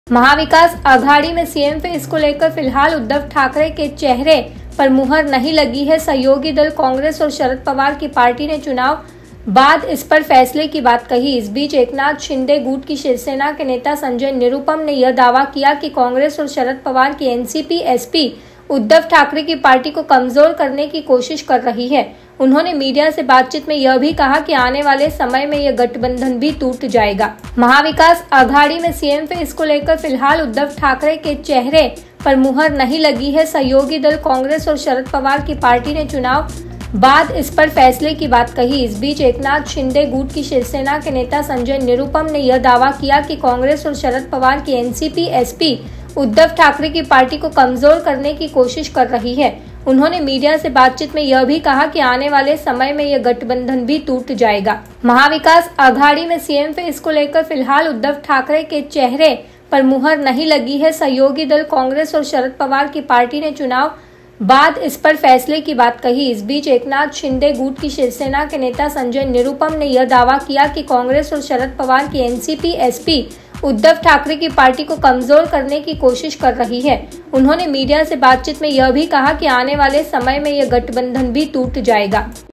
Sanjay Nirupam ने किया बड़ा दावा, सीएम फेस को लेकर उद्धव ठाकरे के चेहरे पर मुहर नहीं लगी है
महाविकास अघाड़ी में सीएम फेस को लेकर फिलहाल उद्धव ठाकरे के चेहरे पर मुहर नहीं लगी है. सहयोगी दल कांग्रेस और शरद पवार की पार्टी ने चुनाव बाद इस पर फैसले की बात कही है. इस बीच एकनाथ शिंदे (Eknath Shinde) गुट की शिवसेना के नेता संजय निरुपम (Sanjay Nirupam) ने यह दावा किया है कि कांग्रेस और शरद पवार की एनसीपी-एसपी उद्धव ठाकरे (Uddhav Thackeray) की पार्टी को कमजोर करने की कोशिश कर रही है. उन्होंने मीडिया से बातचीत में यह भी कहा कि आने वाले समय में यह गठबंधन भी टूट जाएगा.